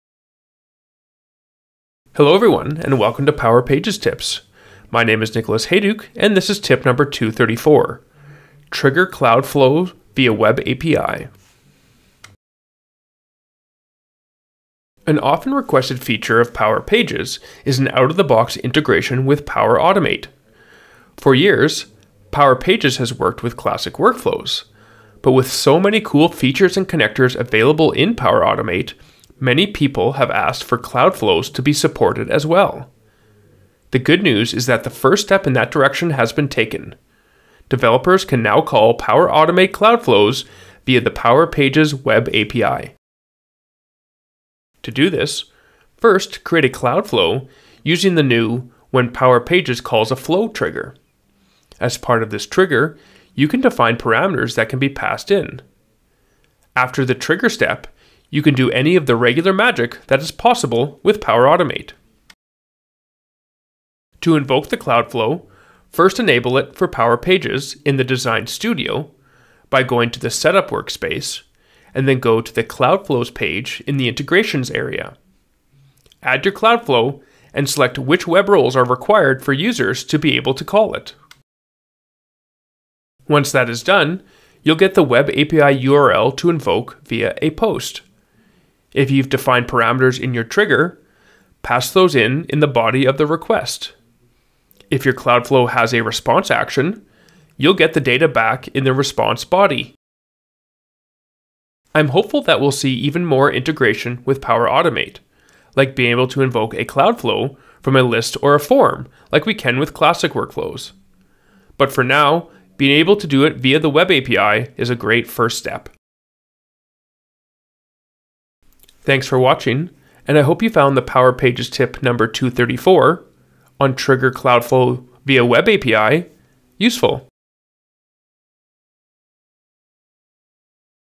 Power Pages Tip #234 - Trigger Cloud Flow via Web API - Engineered Code
In this tip, I look at how you can trigger a Power Automate cloud flow via the Power Pages Web API.